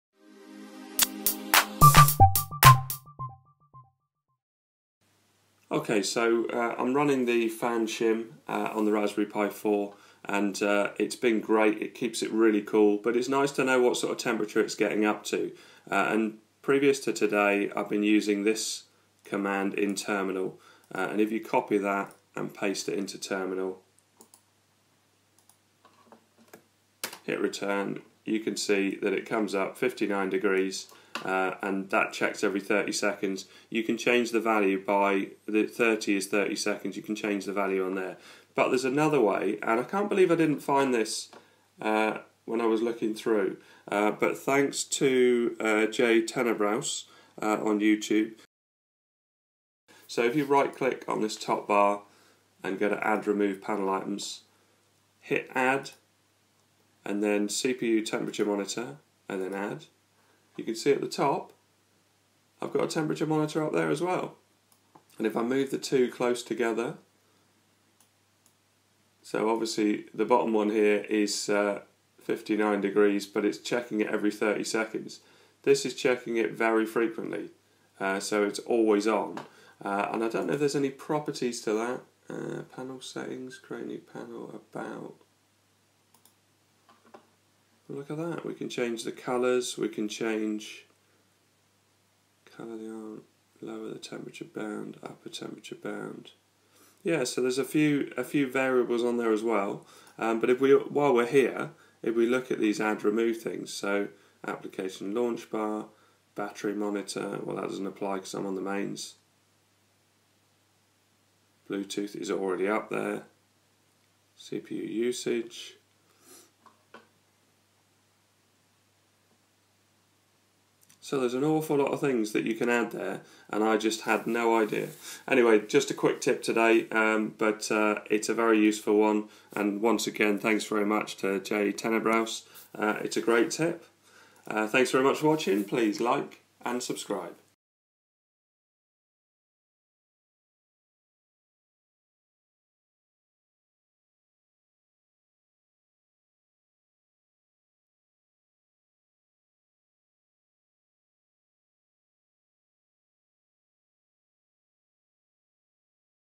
Live Temperature monitor Raspberry Pi 4 Raspbian Overclocked 2.0Ghz
Simple guide to add a live temperature monitor to your Raspbian desktop.

Thanks to
JTenebrous for the tip

CPU Overclocked to 2000Mhz
GPU 600Mhz